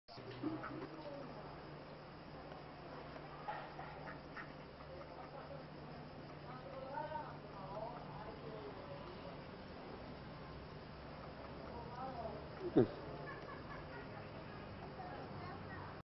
federica fuga dalla papera
giardini di Villa Niscemi (PA) la piccola federica ci ripensa e scappa dalla papera assassina..!!